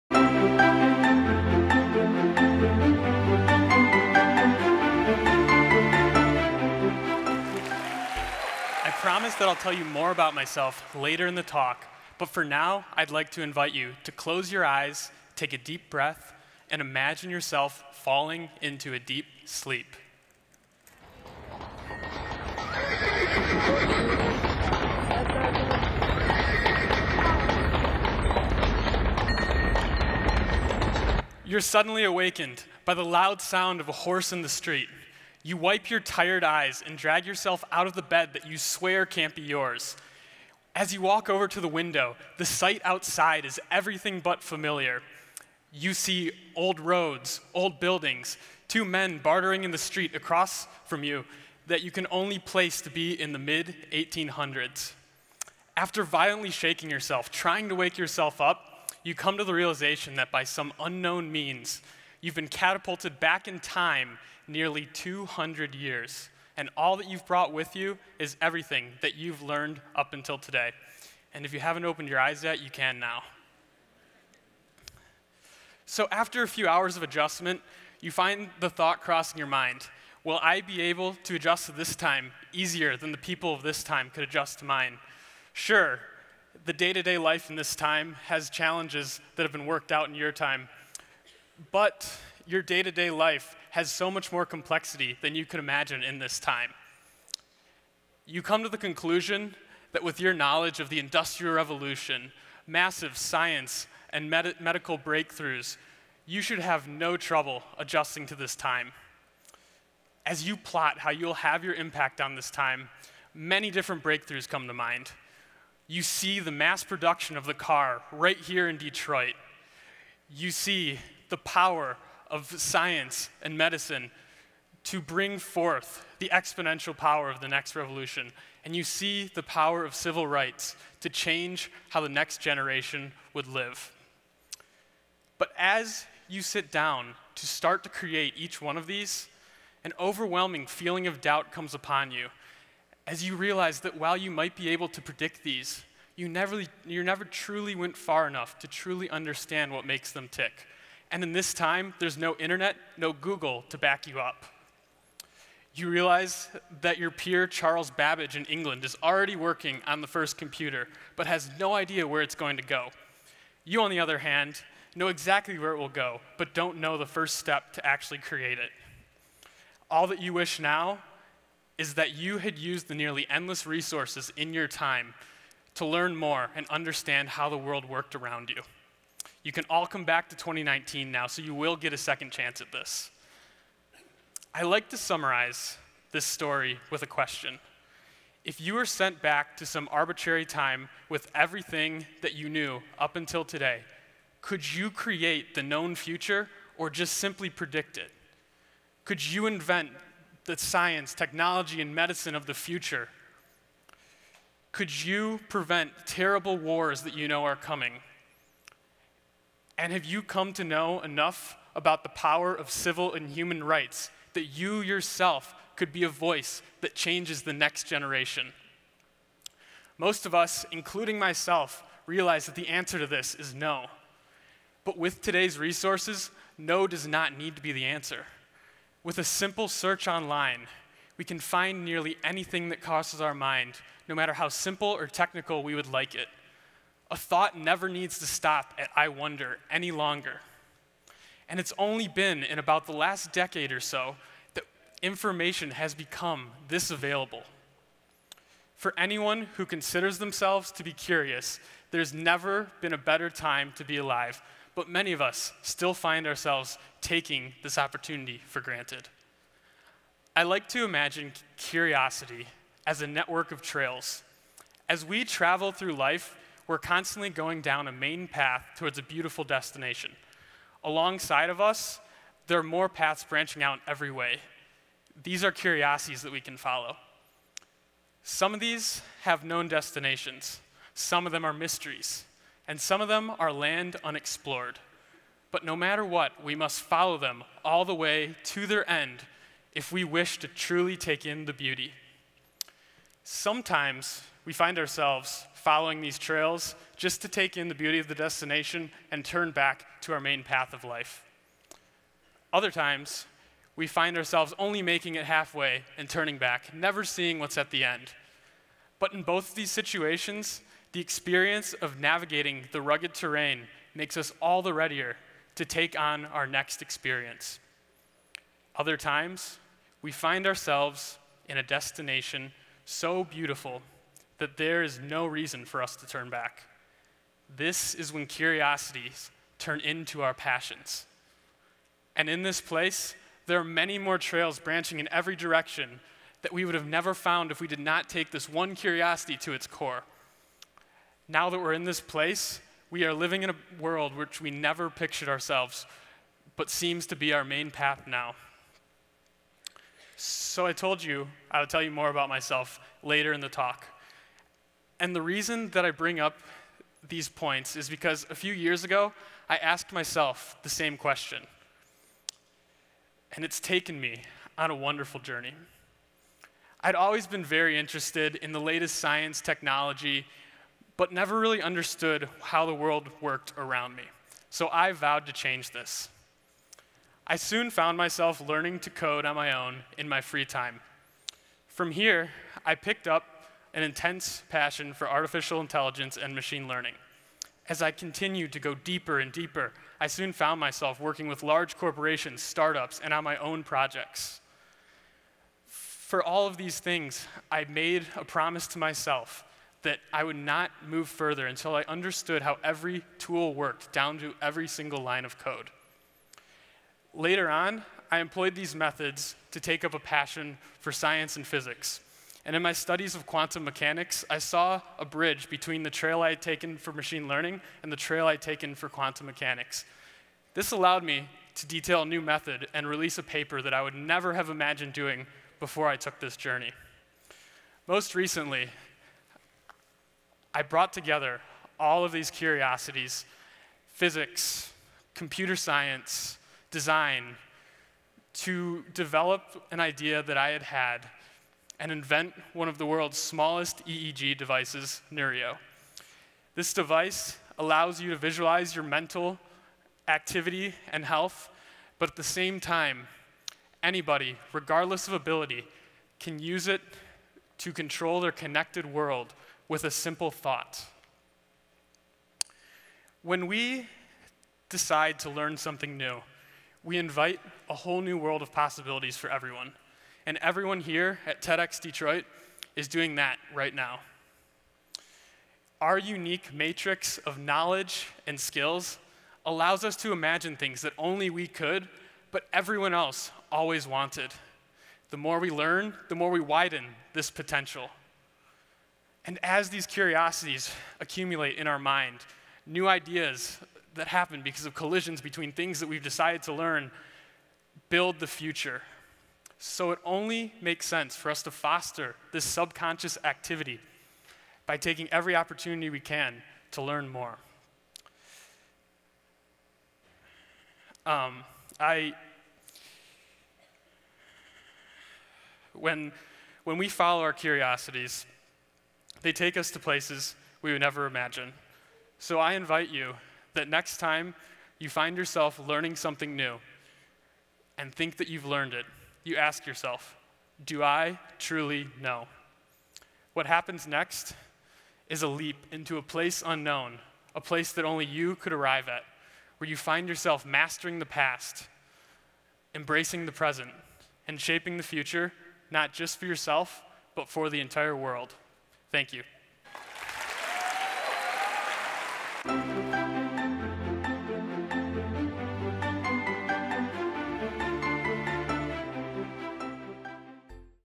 Do You Truly Know? - Curiosity of Today | Ian Rowan | TEDxDetroit
If you were sent back in time with everything you knew up until today, would you have learned enough to shape the known future? Ian takes us on a journey back in time to answer this question and frames a new vantage point for the power of curiosity with today's resources. This inspiring talk leaves the listener with a call to action to master the past, embrace the present, and shape the future.
Ian is the founder and inventor of one of the world’s smallest EEG wearable devices, nurio. Before and since graduating college in 2018, Ian has worked on numerous emerging technologies and has a deep curiosity for research in Science, Technology, Economics, and Philosophy. Ian currently operates MindBuilder AI which conducts research in Artificial Intelligence and builds solutions for businesses around the world.  Ian’s mission is to help other’s unlock their curiosity for the world around them and see the potential in themselves to shape their world. This talk was given at a TEDx event using the TED conference format but independently organized by a local community.